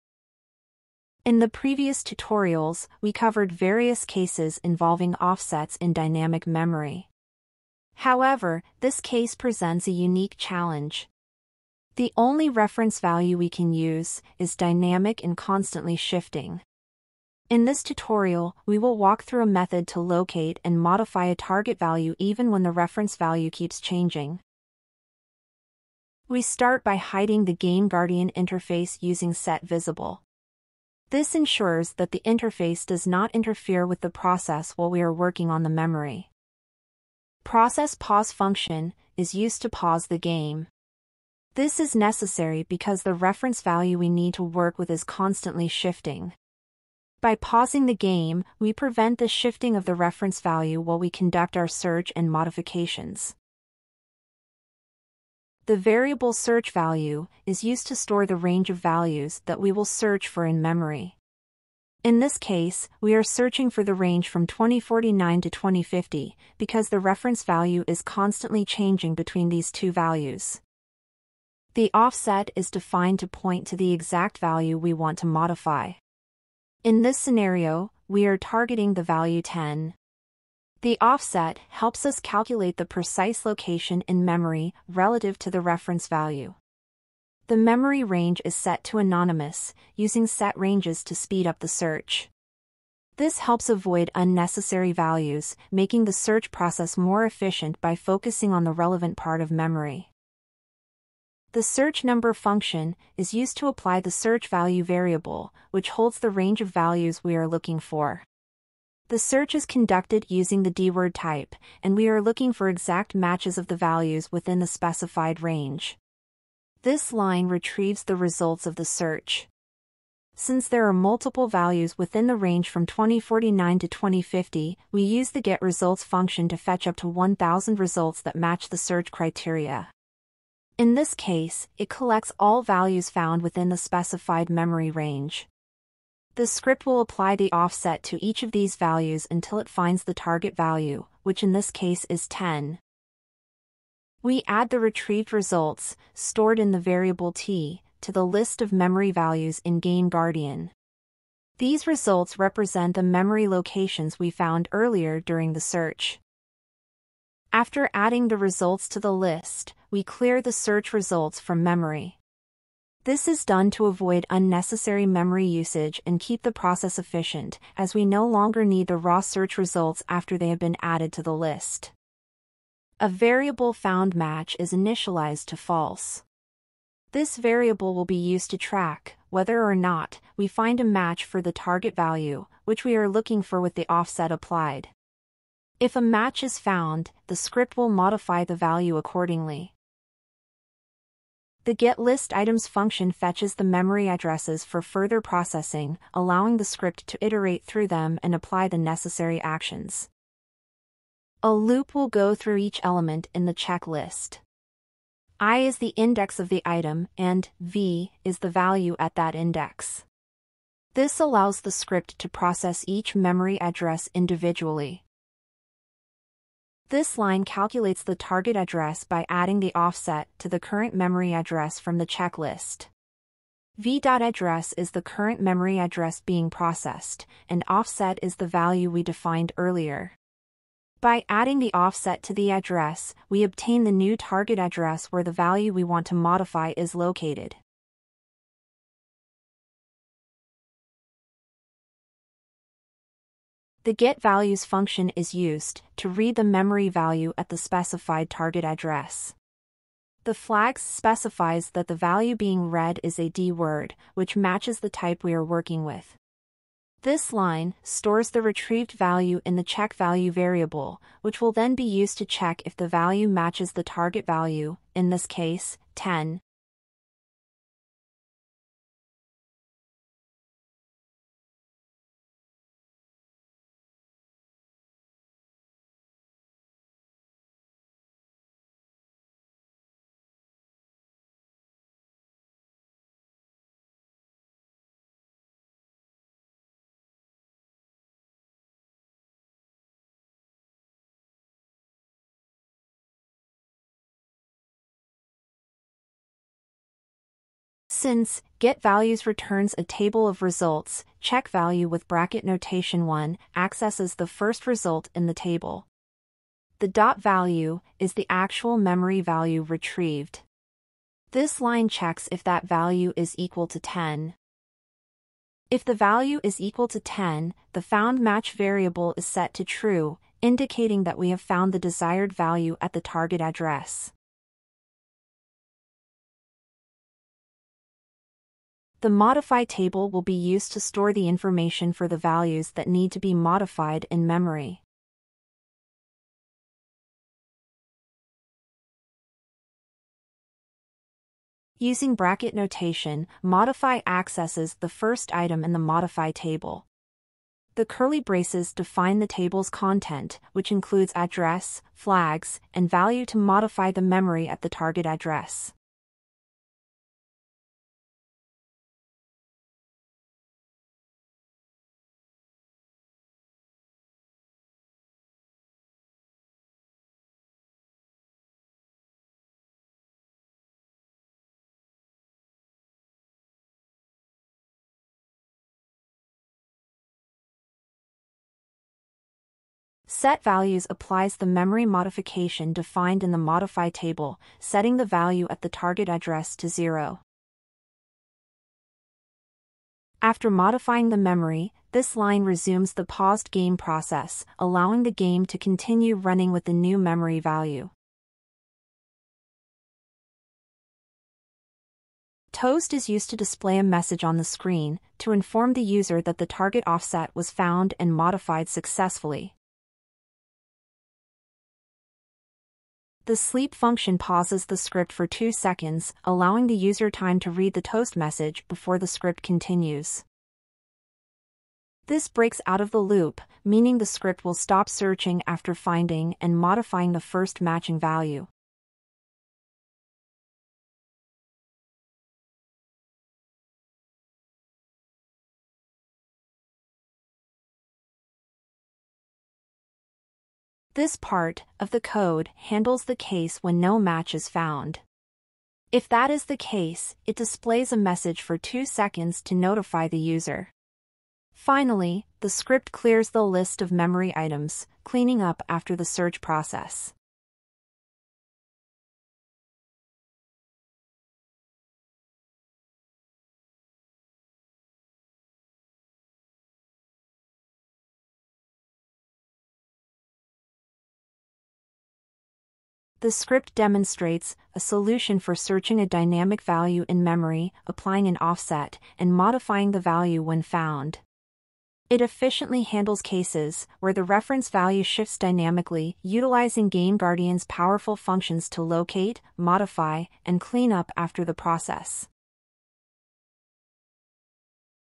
#4 GameGuardian: Auto-Edit DYNAMIC VALUES in ONLINE GAMES
In this tutorial, we dive deep into searching
for dynamic values in memory, applying offsets,
and modifying values efficiently using GameGuardian.
This method is essential for handling cases
where reference values shift constantly,
making static searches unreliable.
The techniques demonstrated are based
on my personal experience with online games
and real-world problem-solving.
Keep in mind that every game is different
and multiple solutions may exist
for the same issue.

and is intended to demonstrate memory
manipulation techniques for research
and learning. I do not endorse or encourage
unethical practices. Always adhere to legal
and ethical guidelines when applying these
techniques.